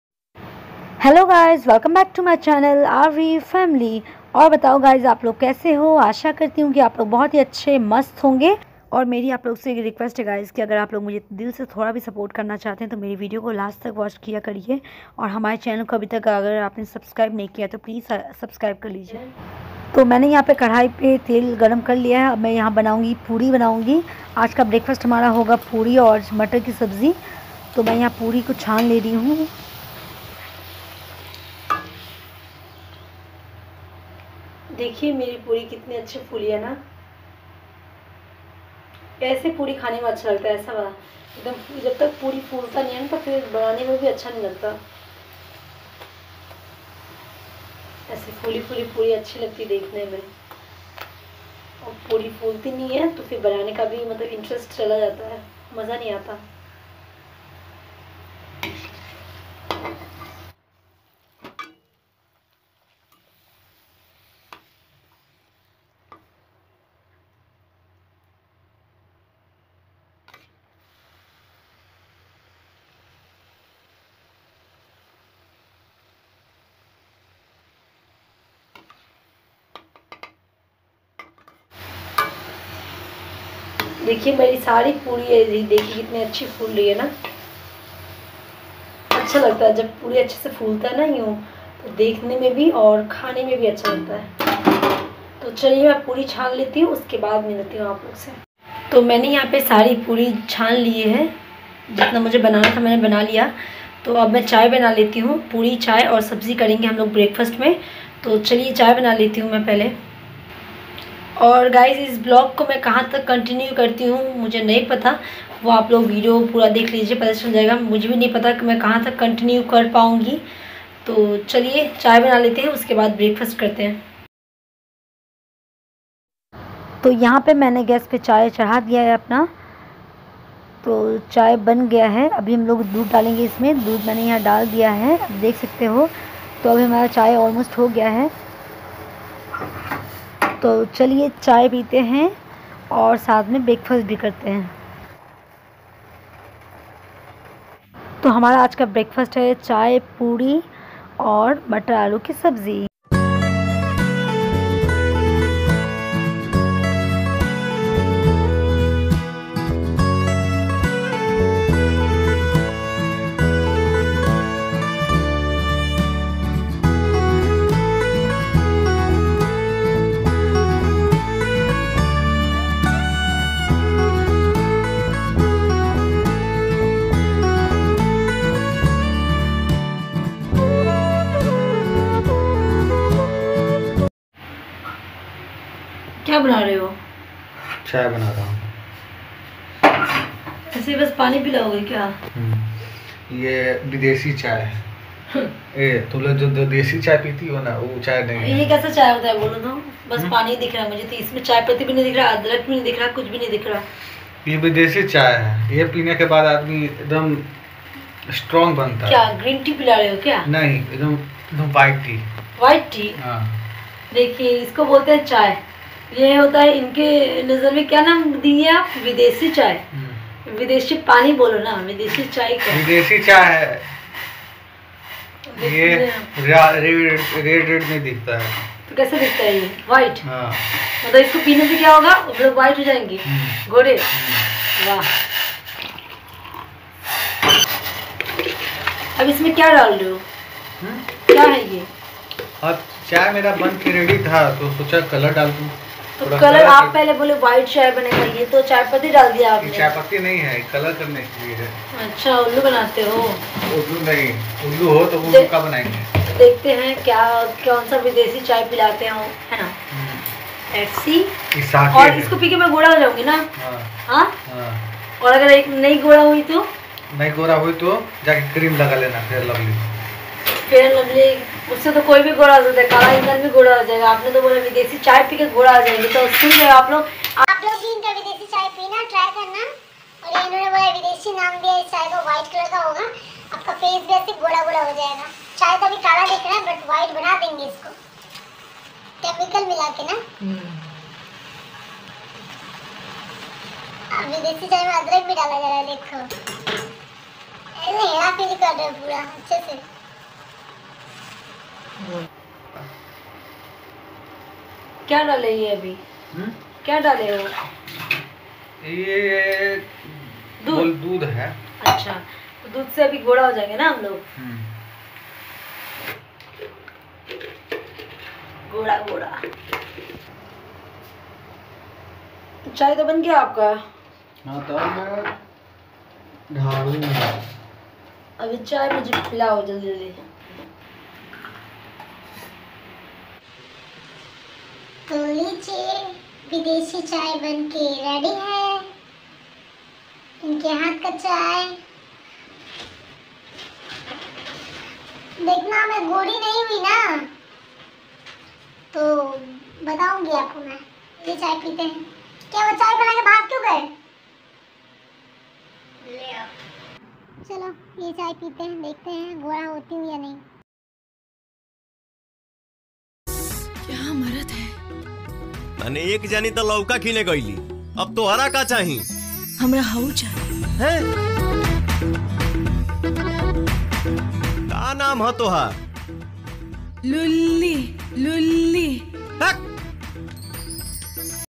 आज मेरे पति बताएँगे गोरा रंग पाने का शॉर्टकट || आइये आप लोग भी चाय पी लीजिये❤|| RVFAMILY
आइये पीते है विदेशी चाय☕

Agar aap mera support karna chahte hai to plz video ko full watch kariyega wahi mere liye sabse bara support hoga..🙏